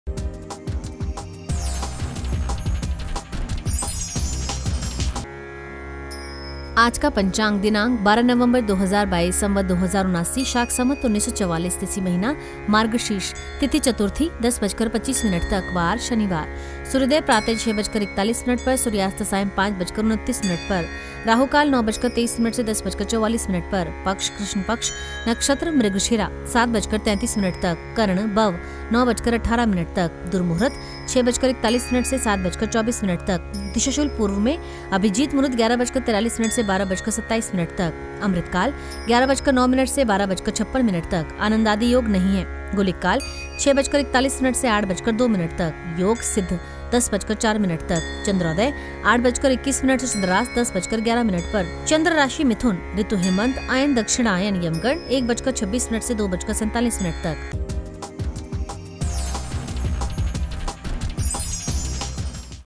Aaj Ka Panchang | 12 नवंबर 2022 | जानें आज का शुभ मुहूर्त और राहुकाल का समय
सनातन पंचांग को वैदिक पंचांग के नाम से जाना जाता है। पंचांग के माध्यम से समय एवं काल की सटीक गणना की जाती है। पंचांग मुख्य रूप से पांच अंगों से मिलकर बना होता है। ये पांच अंग तिथि, नक्षत्र, वार, योग और करण है। यहां हम दैनिक पंचांग में आपको शुभ मुहूर्त, राहुकाल, सूर्योदय और सूर्यास्त का समय, तिथि, करण, नक्षत्र, सूर्य और चंद्र ग्रह की स्थिति, हिंदूमास एवं पक्ष आदि की जानकारी देते हैं। आइए जानते हैं आज का शुभ मुहूर्त और राहुकाल का समय।